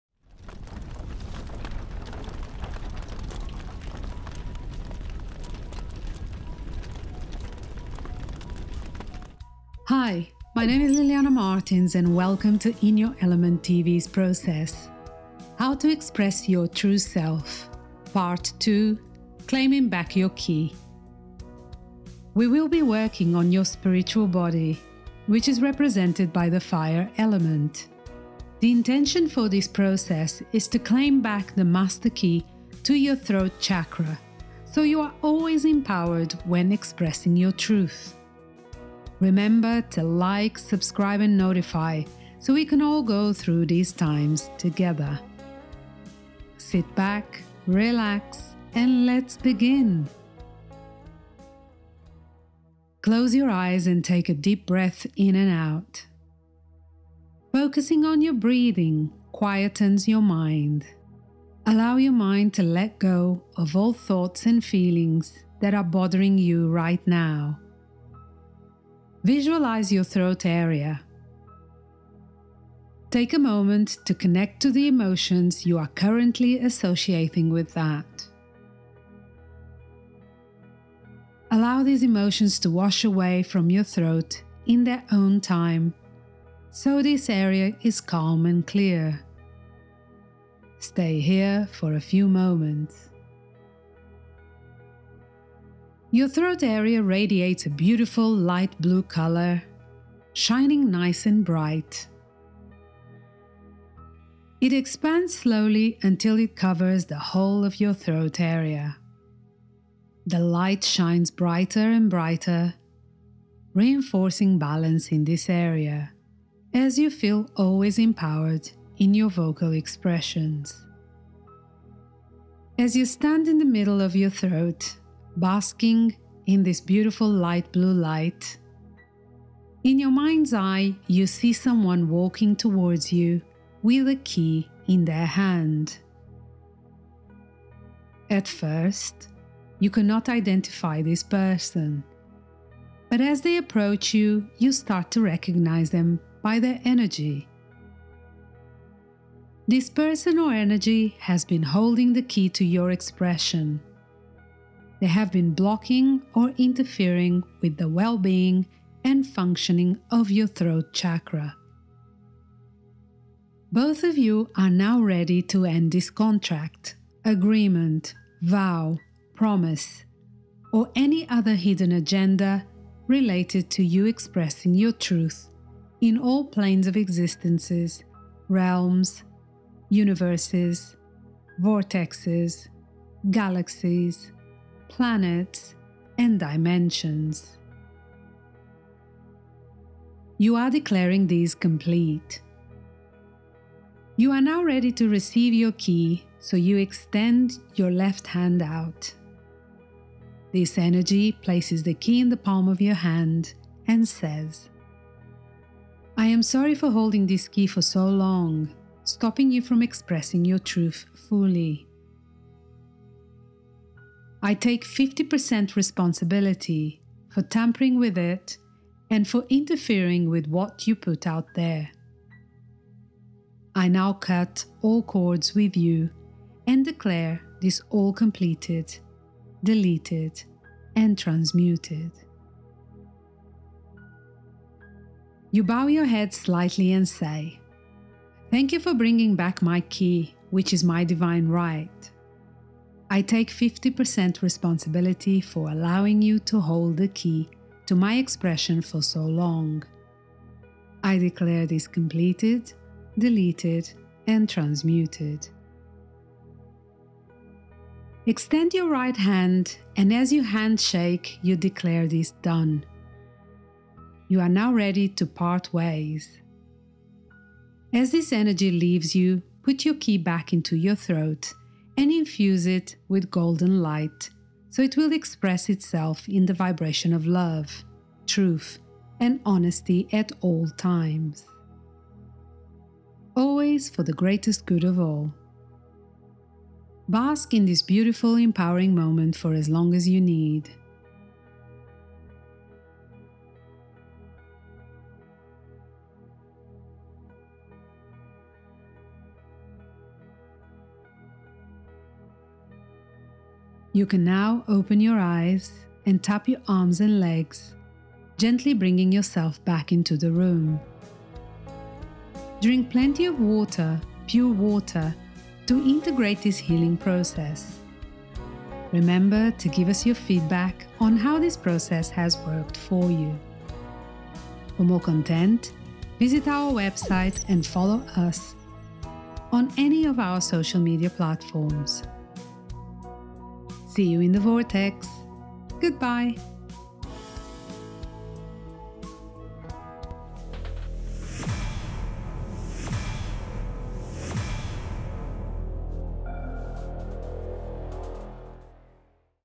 How to express your true self - part 2 | 432hz Music |IN YOUR ELEMENT TV
💙 This process helps to clear your solar plexus area from negative energies and restore your true self energy here for clearer, transparent and empowered communication.

💛 In this video you will get the master key to your solar plexus chakra's energy and so you can unlock your true expression and communicate freely and truly. Claim your power back and re-invent yourself.

2️⃣0️⃣2️⃣3️⃣ the year of Alchemy